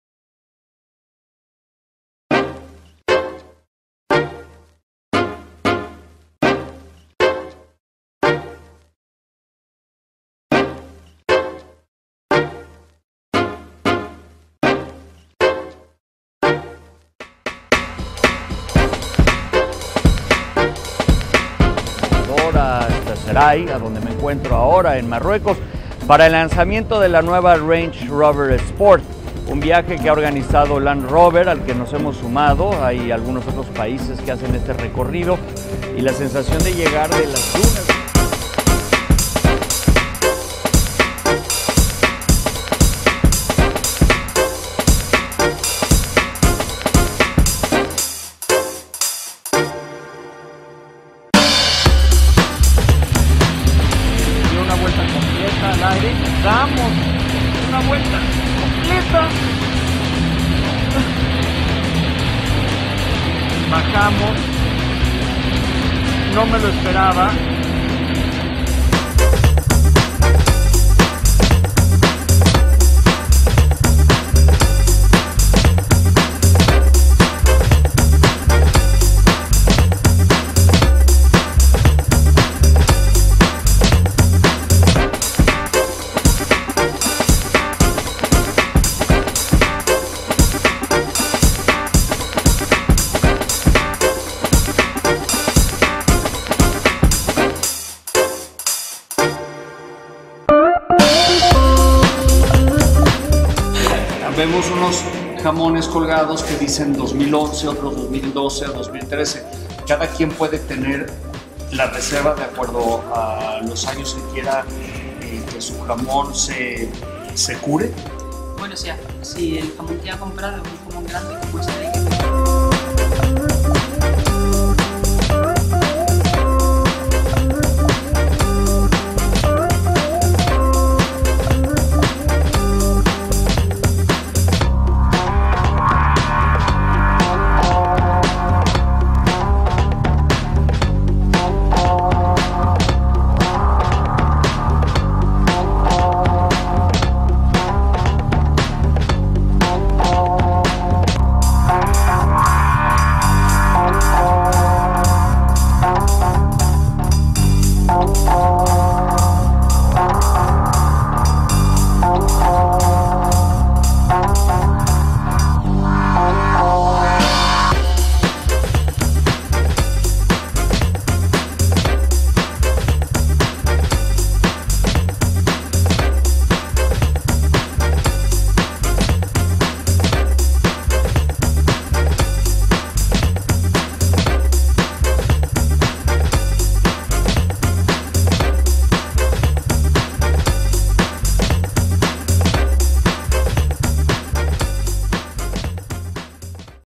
Demo Eddy Warman ¿Y ahora, dónde esta Eddy?
Aquí una copilación de los trabajo y viajes realizados por el periodista Eddy Warman.